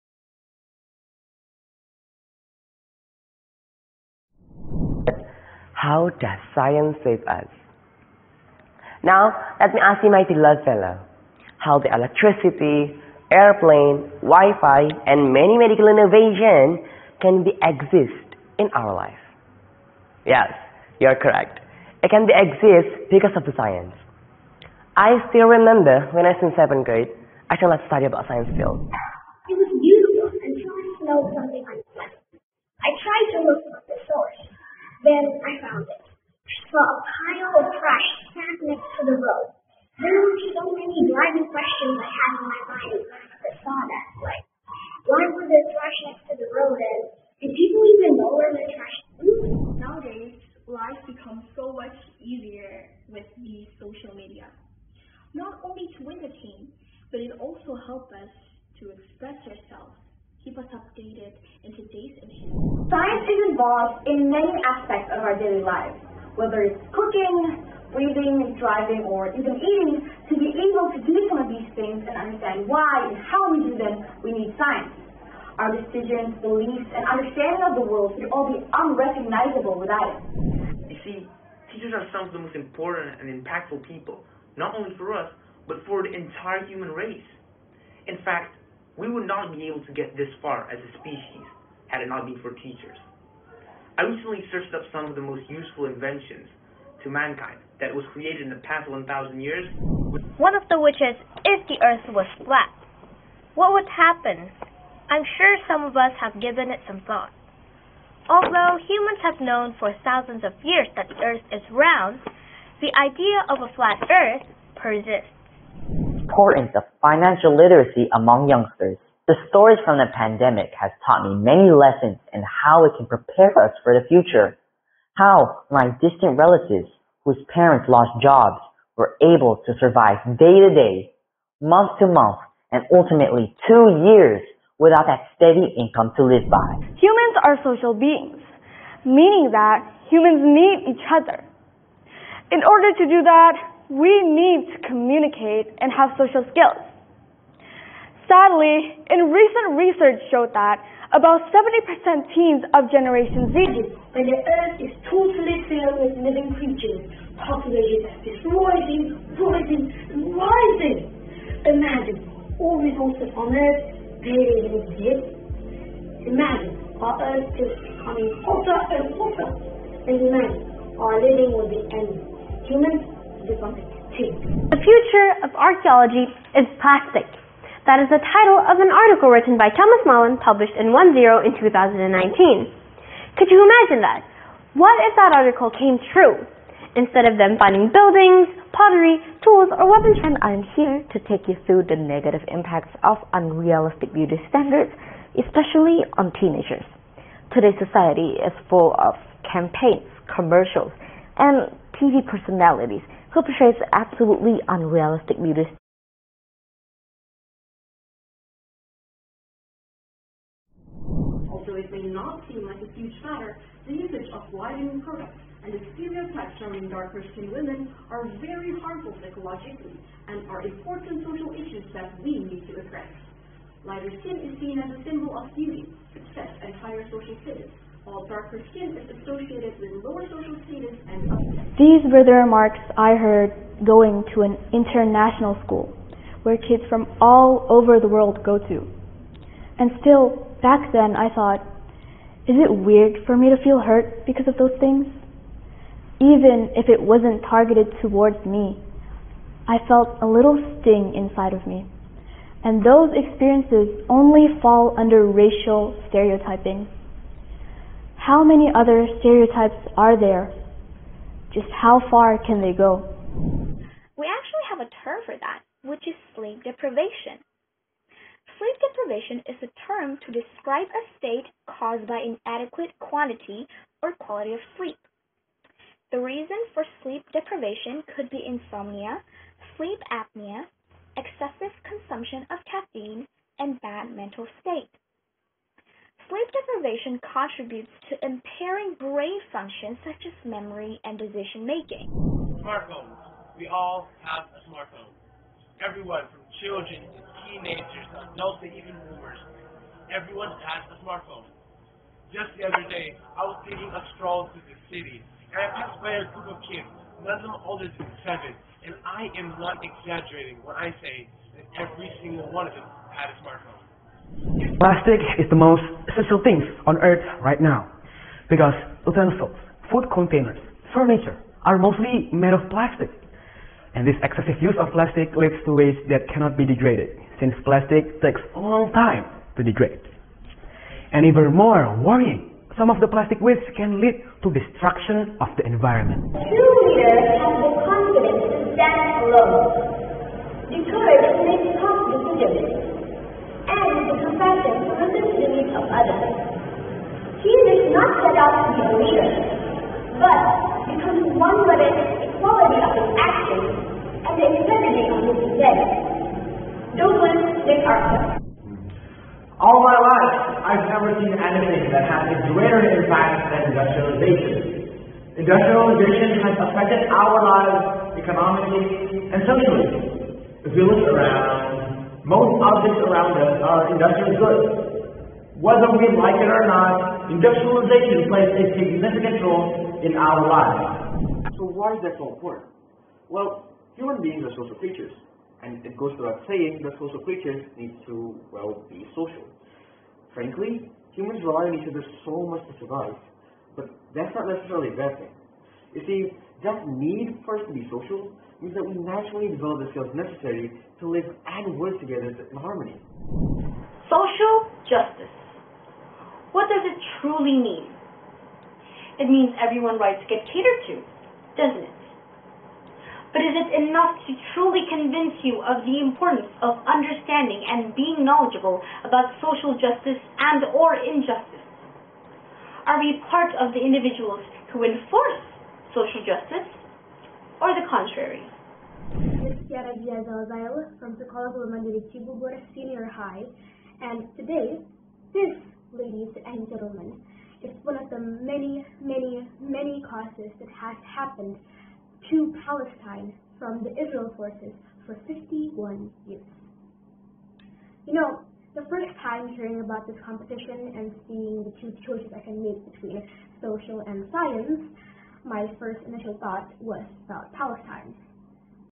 Peserta TOP 21 Speech Contest 2022 Indonesia Science Center
Halo Sobat Sains! Indonesia Science Center sudah melaksanakans seleksi pertama Speech Contest 2021! Ini adalah cuplikan penampilan dari TOP 11 kategori Kids and Teenagers dan TOP 10 kategori Young Adults dari total 507 peserta. Mau tahu sekeren apa penampilan mereka? yuk simak video berikut ini!